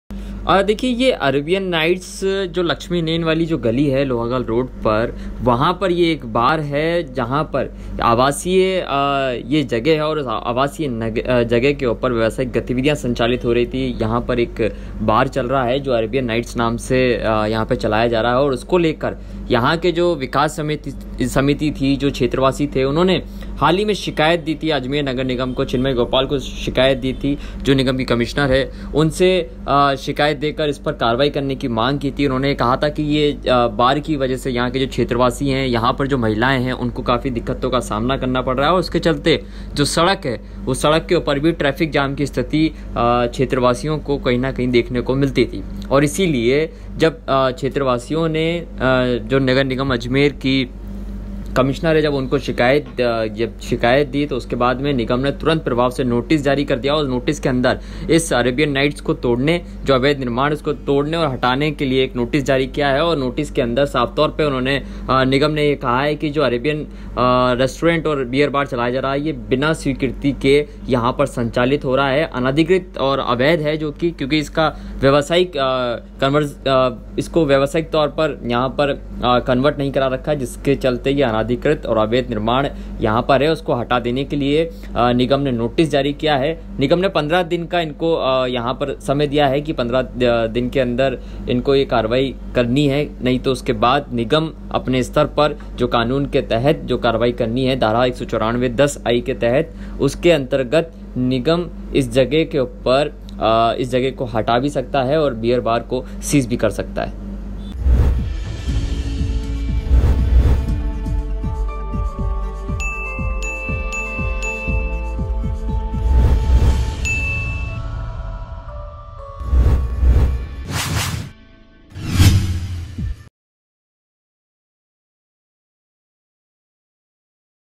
AJMER NEWS | अरेबियन नाइट्स बार को अजमेर नगर निगम ने दिया नोटिस | MTTV INDIA
अरेबियन नाइट्स बार को अजमेर नगर निगम ने दिया नोटिस
आवासीय मकान में हो रही है व्यावसायिक गतिविधि
निगम ने नोटिस में बार को बताया अनाधिकृत
3 दिन में बार को हटाने का दिया नोटिस
क्षेत्रवासियों ने निगम कमिश्नर चिन्मयी गोपाल से बार की समस्या को लेकर दी थी शिकायत